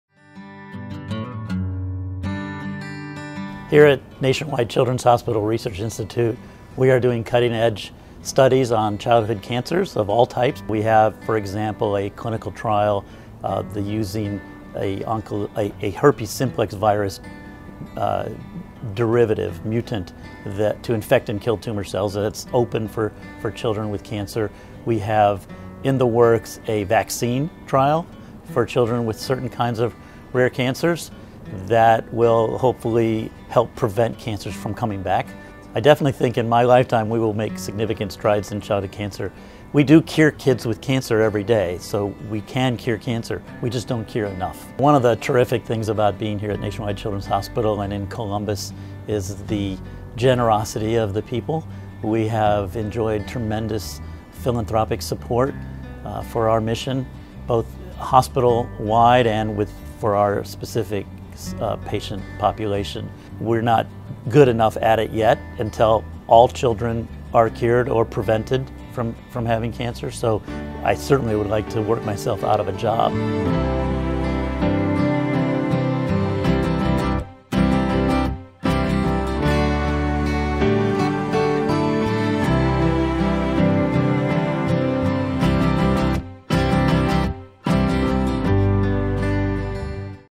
2014 Telethon: Dr. Cripe's work to cure cancer
Every 30 minutes in the United States, a child is diagnosed with cancer. That means during your lunch break, two families were devastated. Dr. Timothy Cripe, chief of the Division of Hematology/Oncology and Bone Marrow Transplant at Nationwide Children's Hospital, is determined to make a difference for those families. His research focuses on developing new treatments for childhood cancer using viruses as an alternative to chemotherapy.